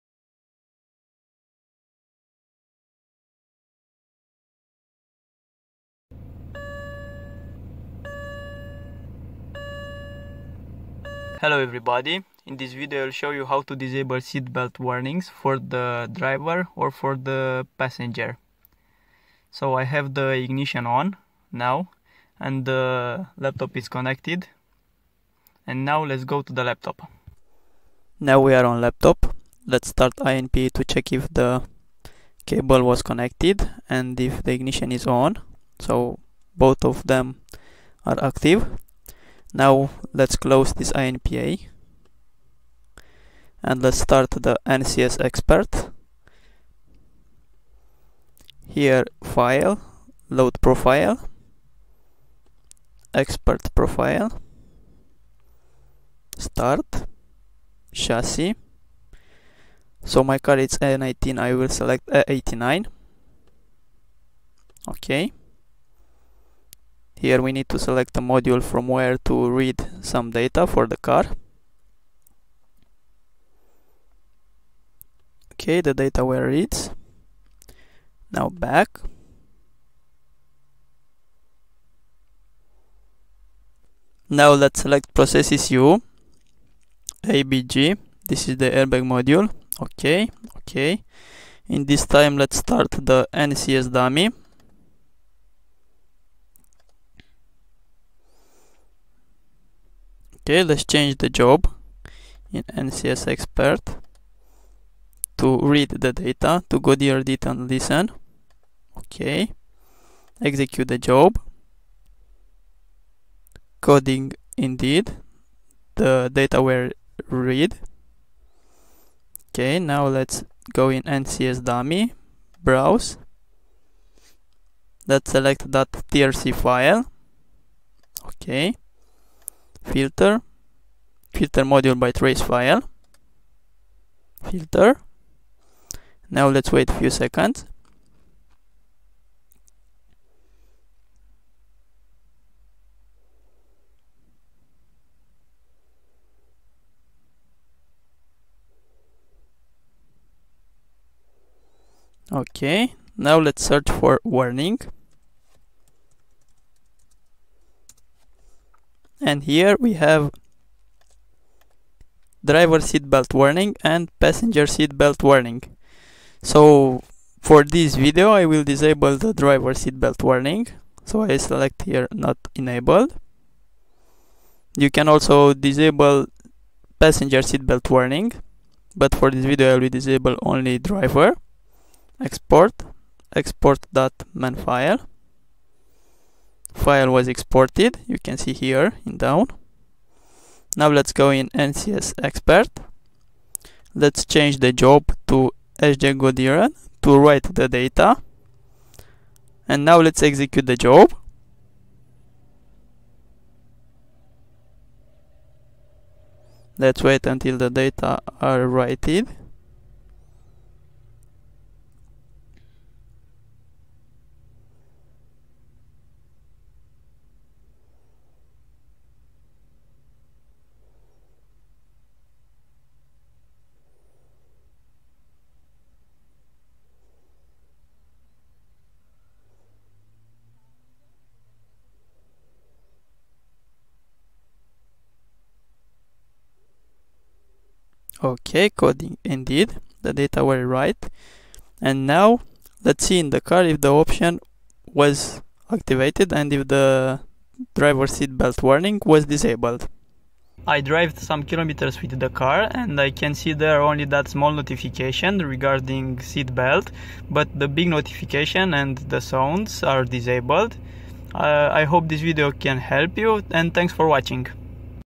8. Disable seat belt warning for Passenger or Driver
In this video you can see how to disable Seat belt Warning for Passenger or Driver on your BMW e90 car.

WARNING.This video is for education purpose. I do not recommend you to disable this feature since wearing of seat belt can save your life.

You need to make 1 change for this option:

1) search for SBR_FAHRER_1 for Driver seat belt and change it to nick_aktiv - disabled
   search for SBR_BEIFAHRER_1 for Passenger seat belt and change it to nick_aktiv - disabled

The same procedure can be used to code BMW E81, E82, E84, E87, E88, E89, E90, E91, E92, E93

For a detailed video about the programming, please see my video: 2. Activate US lights on BMW e90

WARNING
You are doing this on your own risk.
I'm not responsible for any damage on your car.
I'm not responsible if this option is no legal in your country.